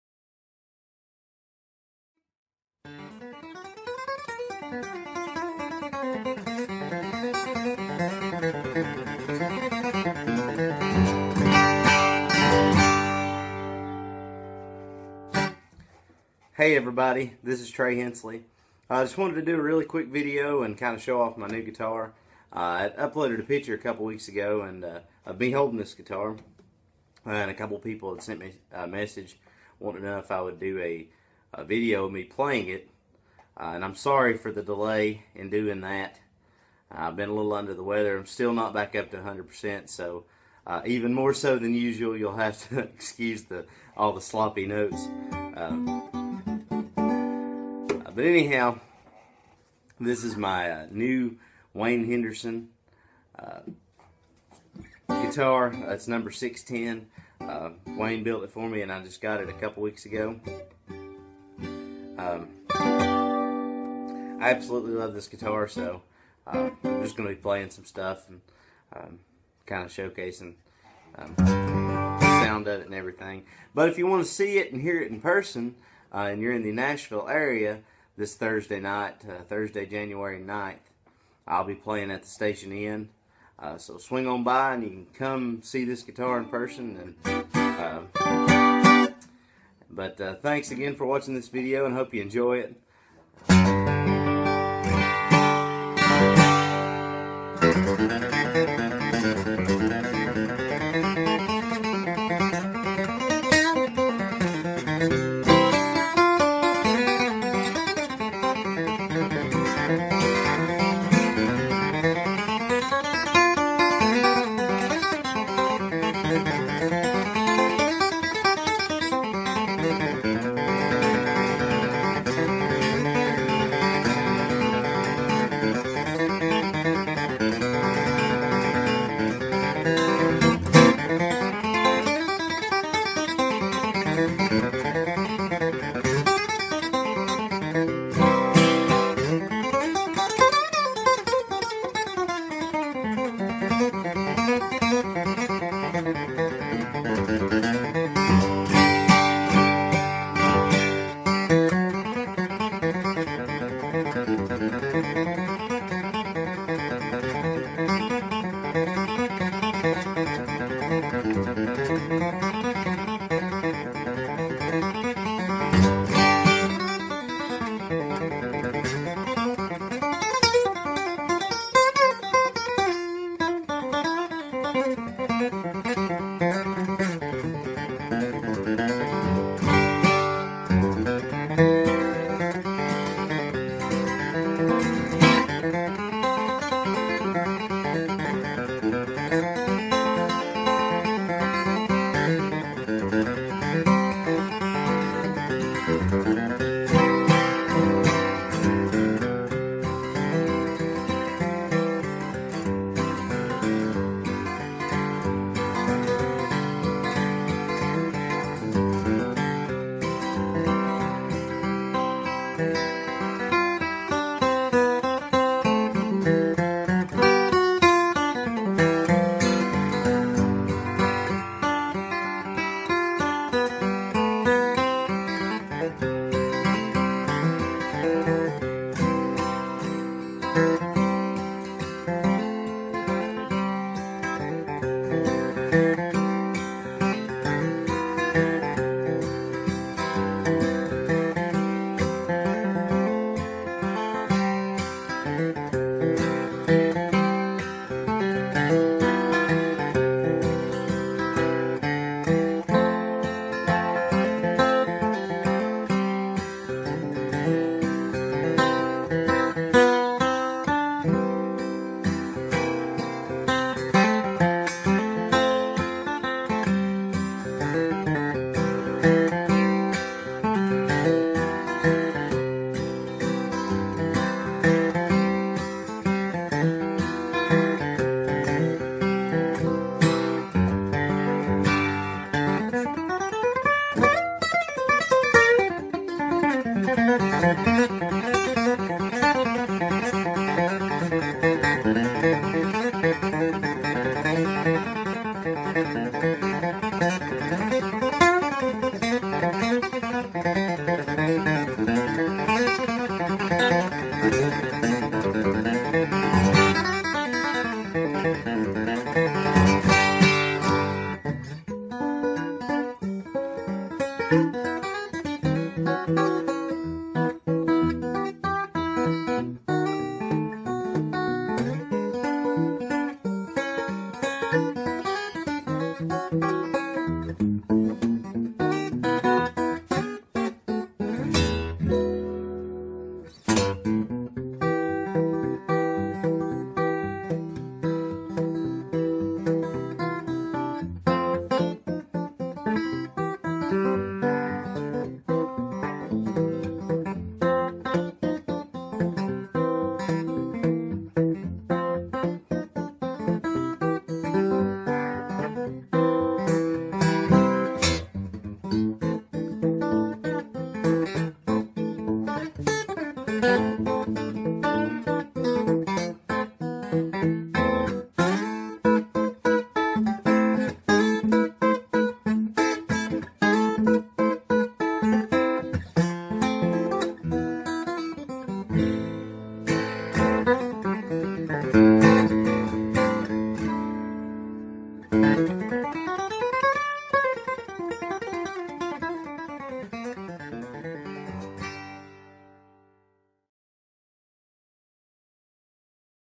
Trey Hensley and the new Wayne Henderson #610
I had several people wanting to know if I would do a video of me playing my new Wayne Henderson guitar (#610) that Wayne built for me...so, here you go!  Please excuse the sloppy playing!